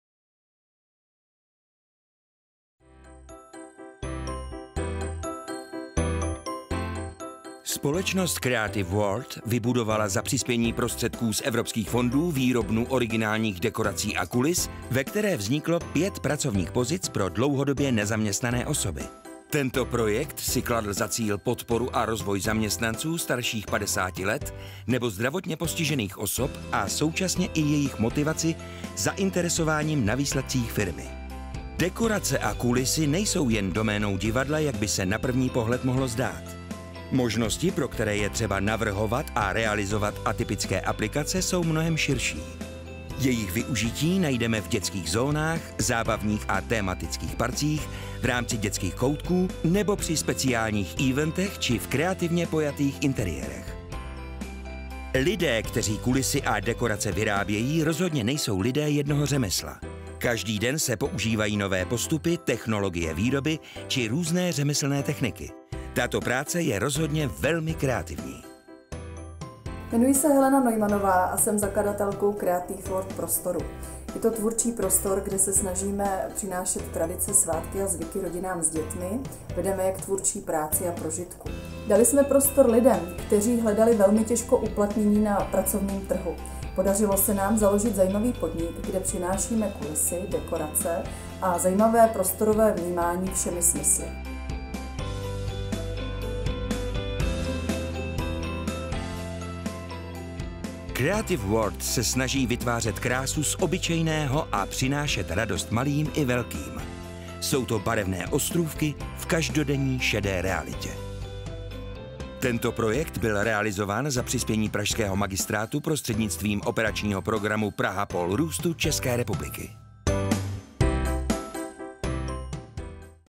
Výroba unikátních dekorací a kulis
Díky nově vytvořeným aktivitám v sociálním podniku Creative word, s. r. o. je zapojeno minimálně 5 osob z cílové skupiny osob dlouhodobě nezaměstnaných, nezaměstnaných starších 50 let nebo občanů se zdravotním postižením do výroby unikátních dekorací a kulis. V rámci daného podnikání se organizace věnuje rozvoji zaměstnanců, jejich motivaci a vlastní zainteresovanosti na výsledcích podniku. Možností využití dekorací a kulis, vyrobených cílovou skupinou, je mnoho - od dětských zón, zábavních a tematických parků, zvláštních příležitostí, až po divadelní představení.